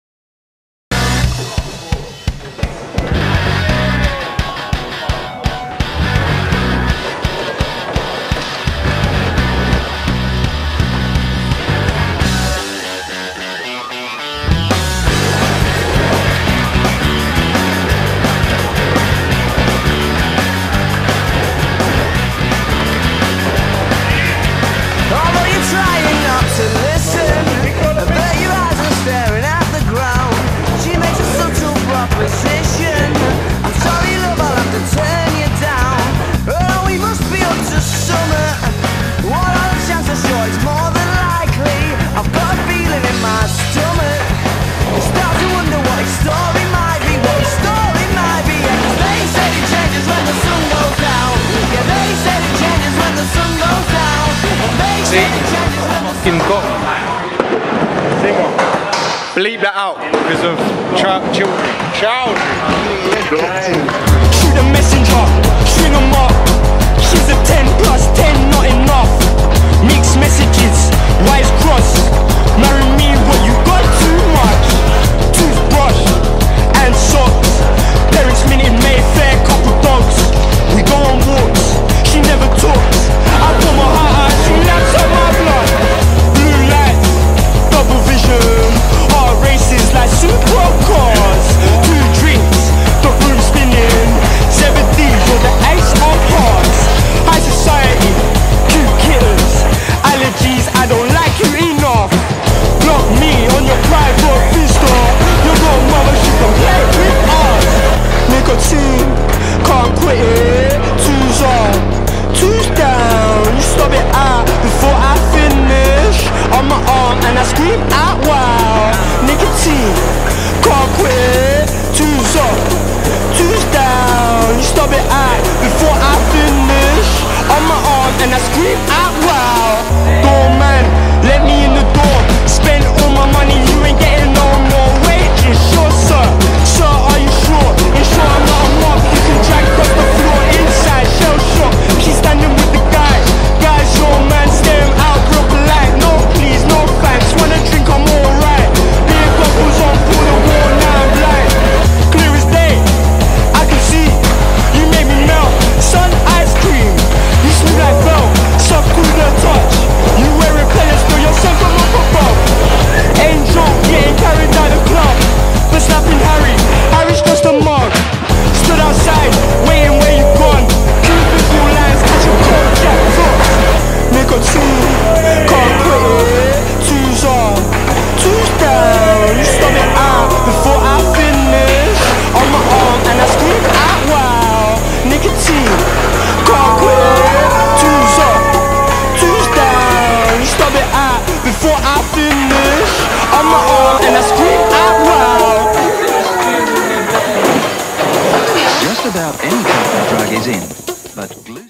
Biscuit Boyzz Takeover
We was invited down to a private mini ramp session with @schoolofskateuk and few of the boys.

Big love to the @Schoolofskateuk, check out their page for some skate coaching and private lessons. Great for the little ones to get involved into the skate lifestyle.